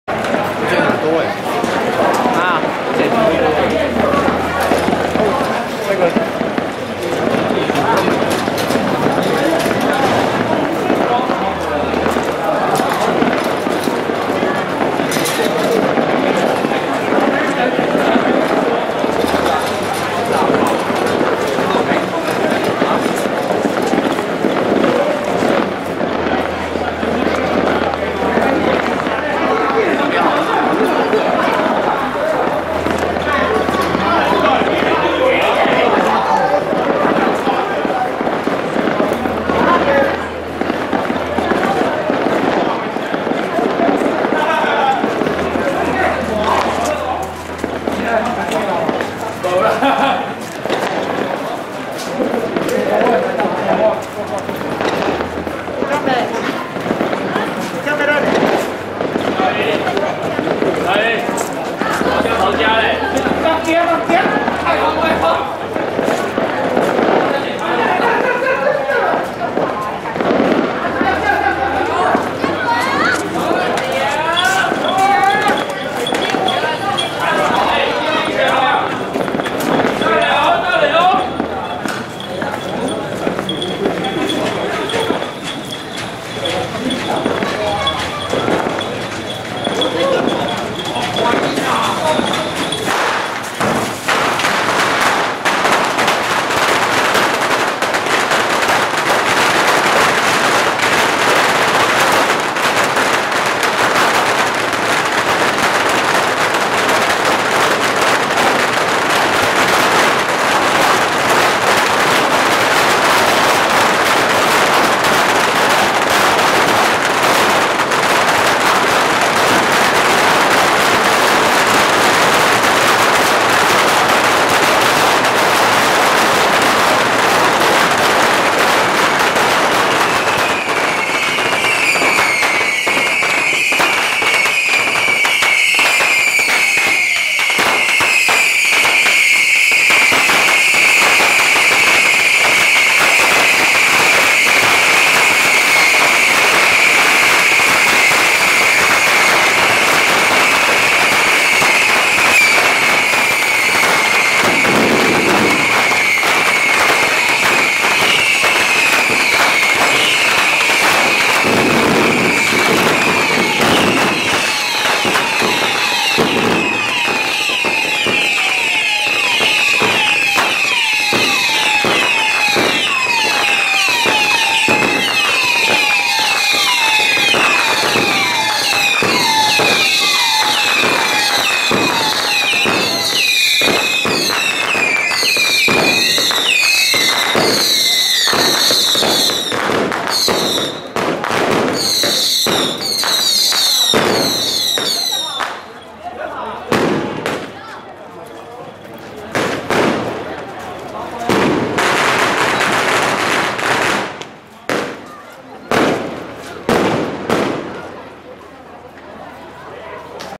2023.02.05《鹽水元宵蜂炮》《李拜六 炮城》
月港後厝仔福安宮廟埕施放